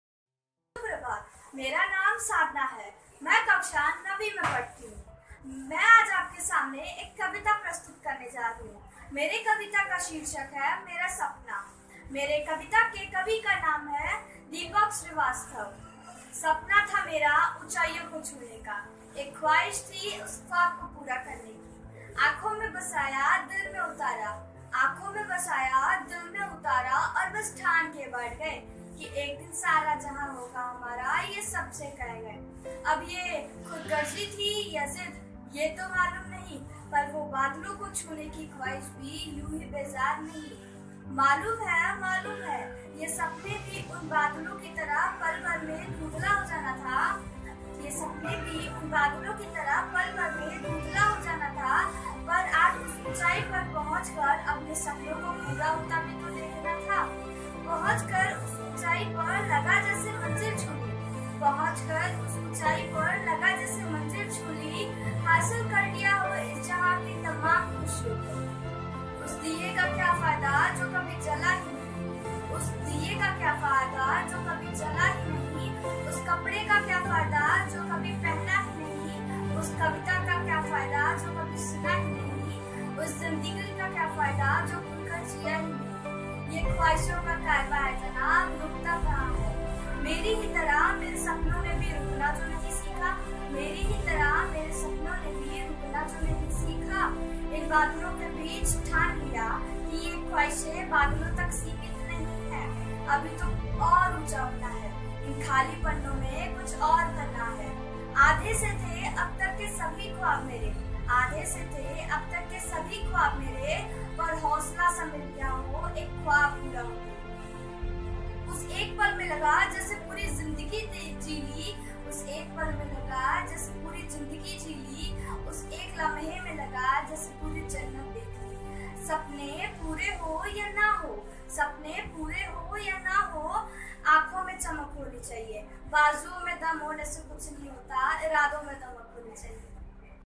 Hindi Poem: Sadhana Pandey, Class 9, APS Dinjan
Army Public School, Dinjan Hindi Poem Recitation Competition - Online Entry